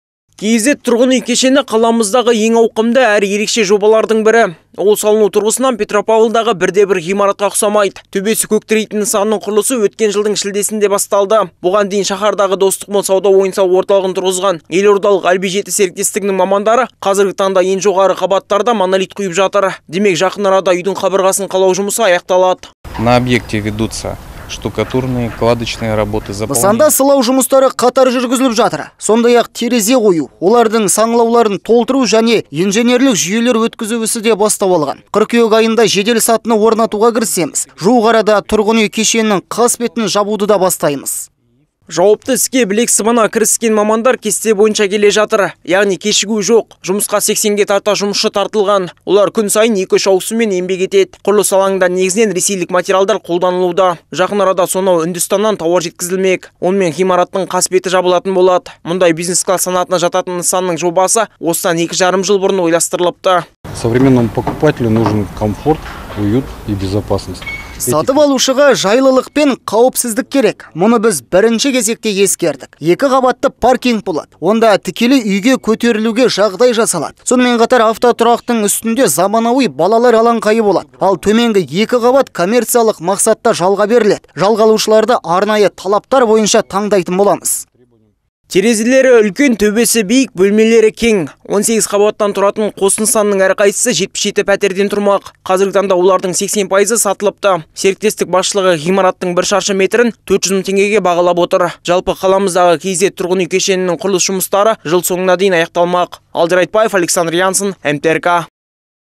"KZ" тұрғын үй кешенінің құрылысы жыл соңында аяқталады
"KZ" тұрғын үй кешені - қаламыздағы ең ауқымды әрі ерекше жобалардың бірі. Ол салыну тұрғысынан Петропавлдағы бірде-бір ғимаратқа ұқсамайды. Төбесі көк тірейтін нысанның құрылысы өткен жылдың шілдесінде басталды. Бұған дейін шаһардағы "Достық мол" сауда-ойын-сауық орталығын тұрғызған елордалық "Альбе-7" серіктестігінің мамандары қазіргі таңда ең жоғары қабаттарда монолит құйып жатыр. Демек, жақын арада үйдің қабырғасын қалау жұмысы аяқталады.